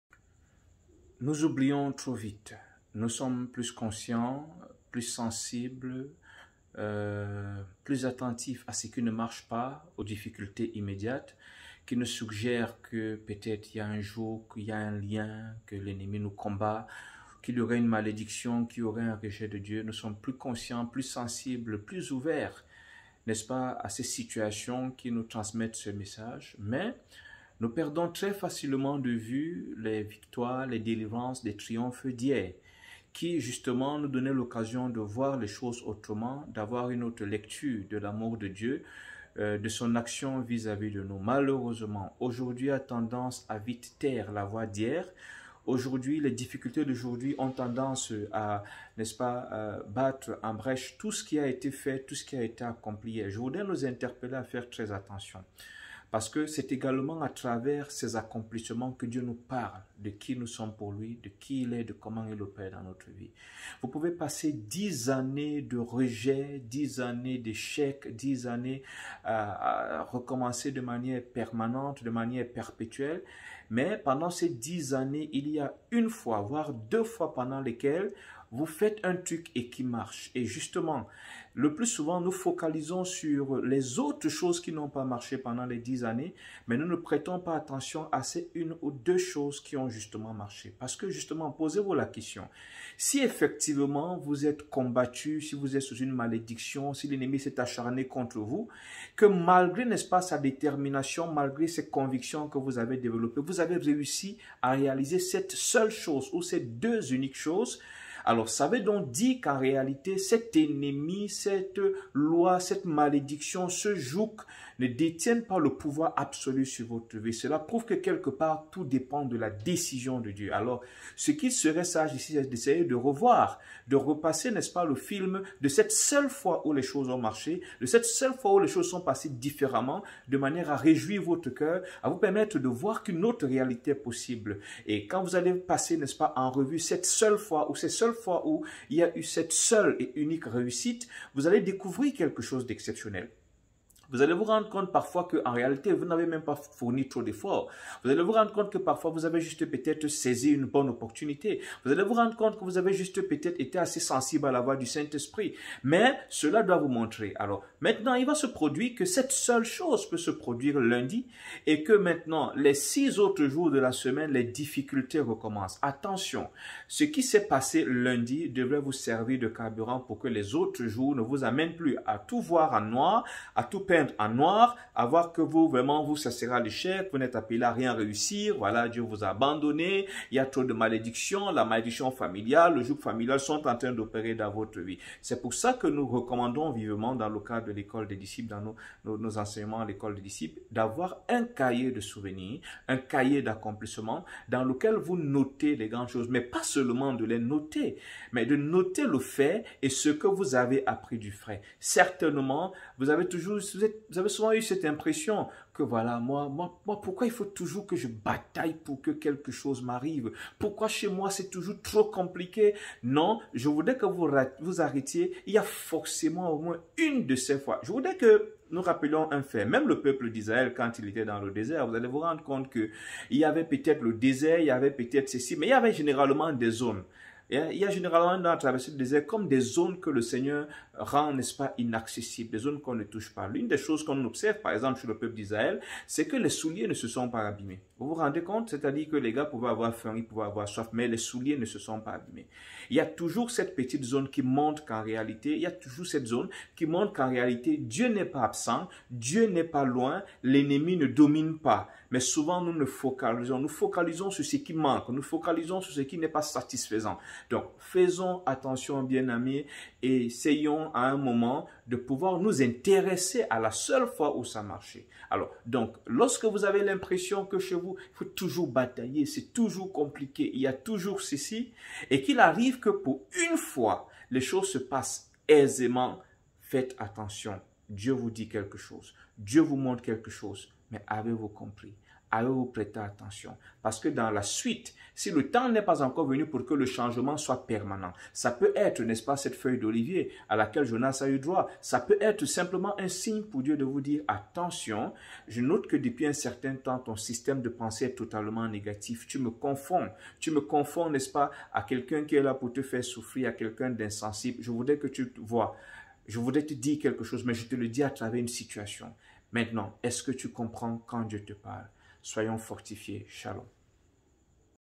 NOUS OUBLIONS TROP VITE !
Rien n'est facile dans ta vie. Il faut toujours beaucoup batailler pour parfois arriver à de rares résultats.
Tu as l'impression que tout semble réuni pour te maintenir sur place...
Voici ce qu'il faut faire